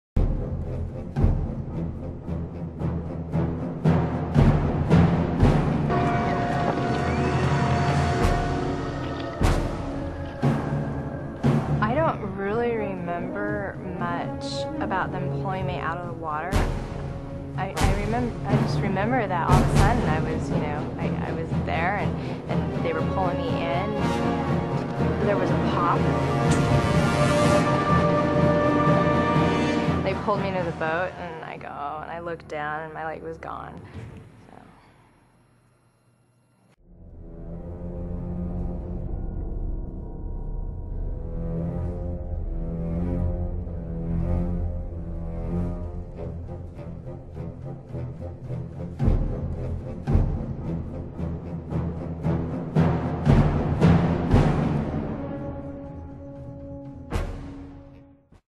Boston Terrier Attack!
A recent attack was caught on film in the waters off Massachusetts...